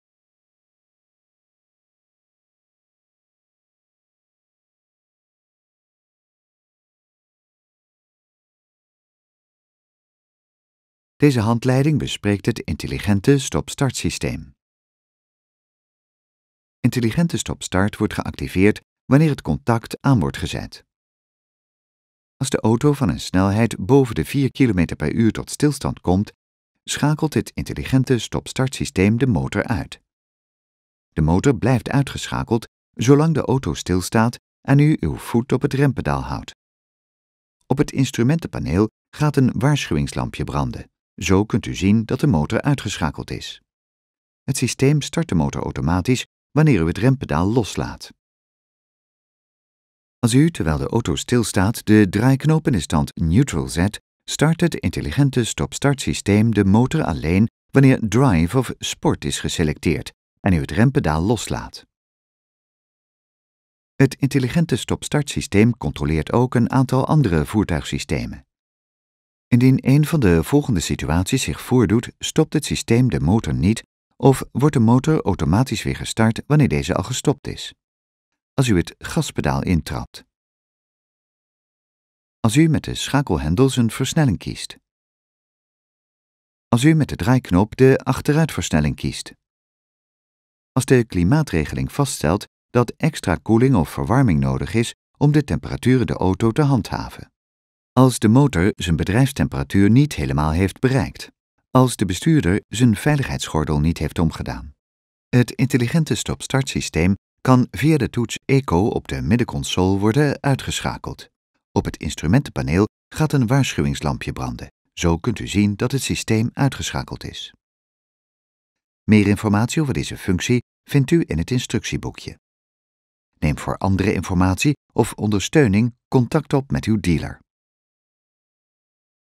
Range Rover Sport 14MY | Intelligent Stop-/Start-systeem
In deze video wordt het intelligent Stop-/Start-systeem van de Range Rover Sport besproken.